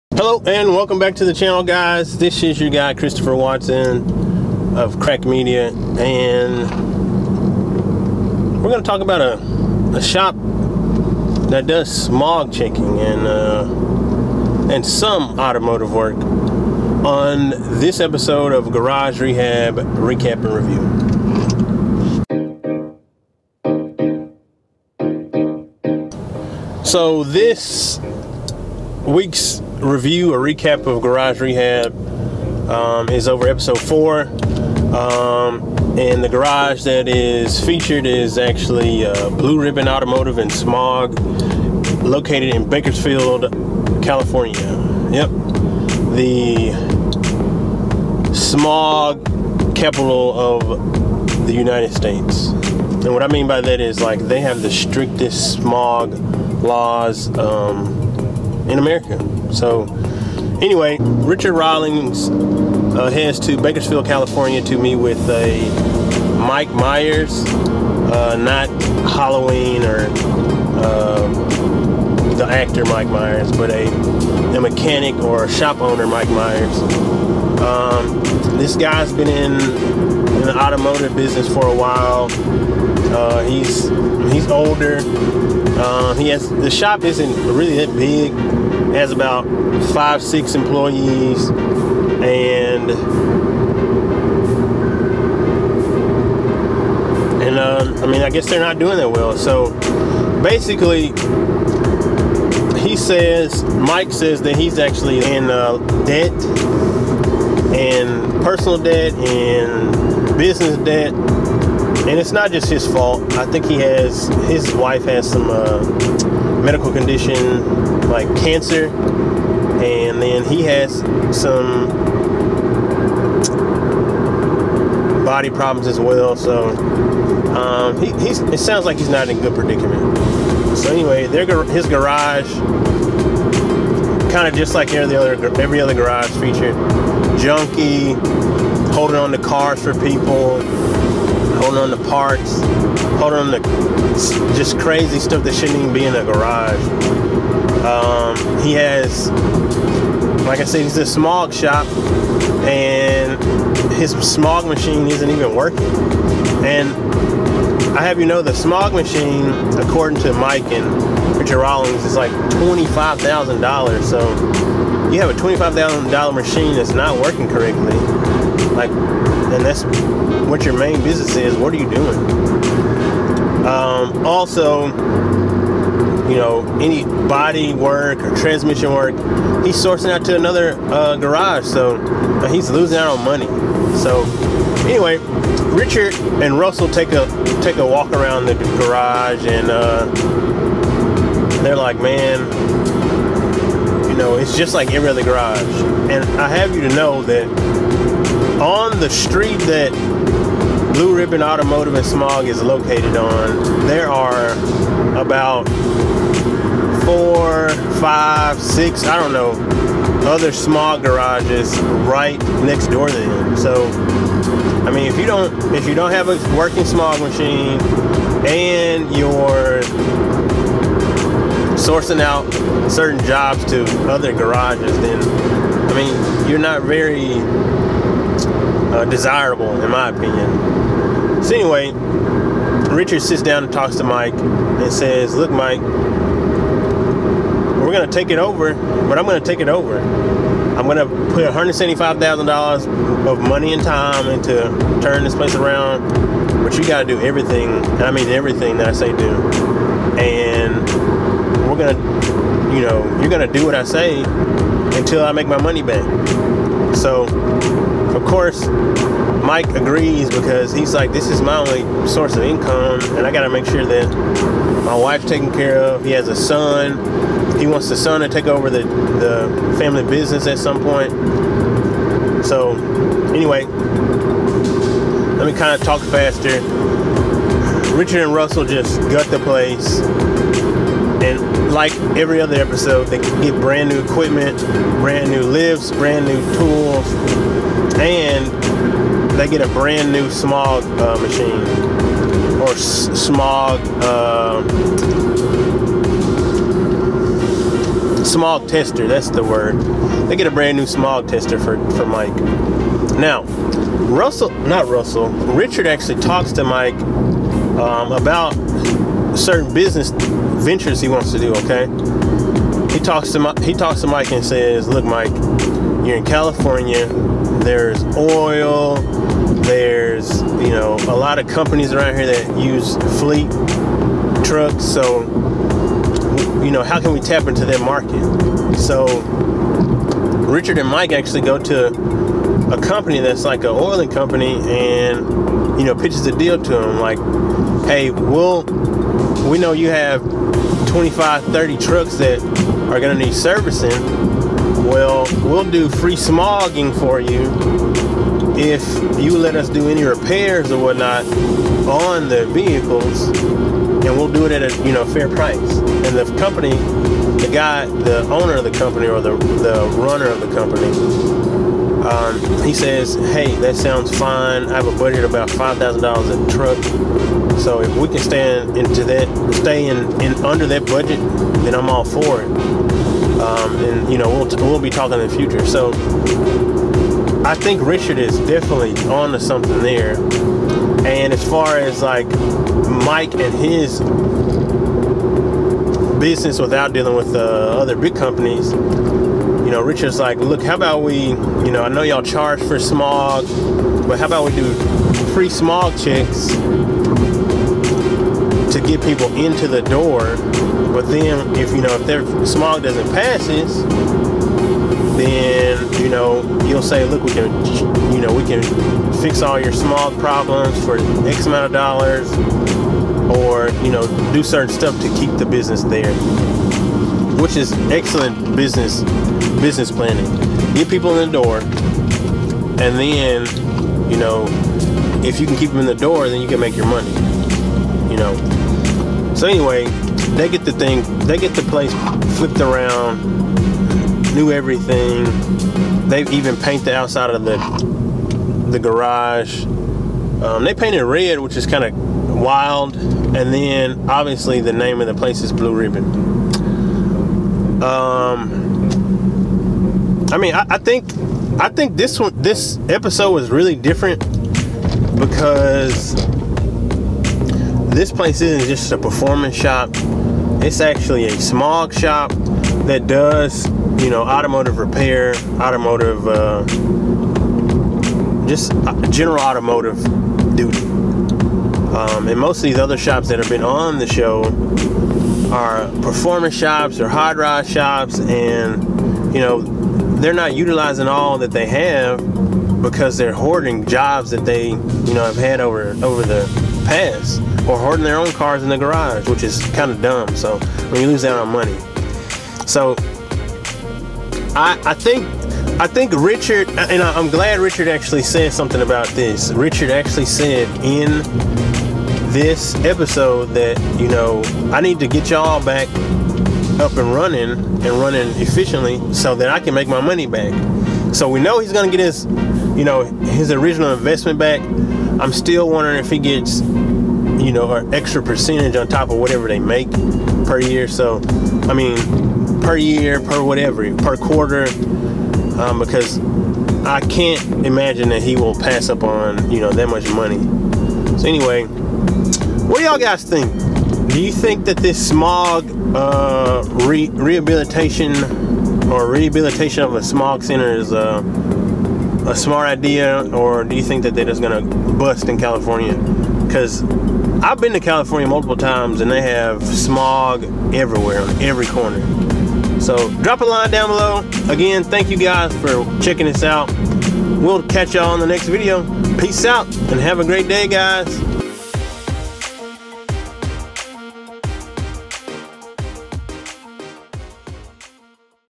Garage Rehab With Richard Rawlings Episode 4 Blue Ribbon Automotive Recap/Review (Discovery Channel)
Garage Rehab With Richard Rawlings Episode 4 Blue Ribbon Automotive Recap/Review (Discovery Channel) - In this episode of Garage Rehab the featured garage is Blue Ribbon Automotive and Smog. Blue Ribbon Automotive is located in Bakersfield, CA. Richard had more to say about business direction in this episode than any other. Hope to see you in the comments.